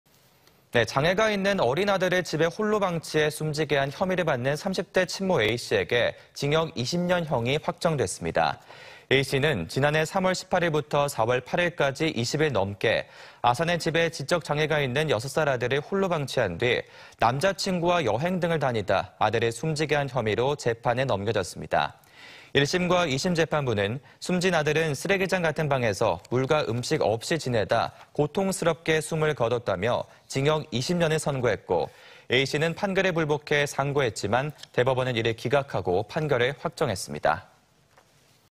6살 지적장애 아들 방치해 숨지게 한 친모, 징역 20년｜ TJB 대전·세종·충남뉴스
장애가 있는 어린 아들을
집에 홀로 방치해 숨지게 한
혐의를 받는 30대 친모 A씨에게
징역 20년 형이 확정됐습니다.

A씨는 지난해 3월 18일부터
4월 8일까지 20일 넘게
아산의 집에 지적 장애가 있는
6살 아들을 홀로 방치한 뒤
남자친구와 여행 등을 다니다
아들을 숨지게 한 혐의로
재판에 넘겨졌습니다.

1심과 2심 재판부는 숨진 아들은
쓰레기장 같은 방에서
물과 음식 없이 지내다
고통스럽게 숨을 거뒀다며
징역 20년을 선고했고,
A씨는 판결에 불복해 상고했지만
대법원은 이를 기각하고
판결을 확정했습니다.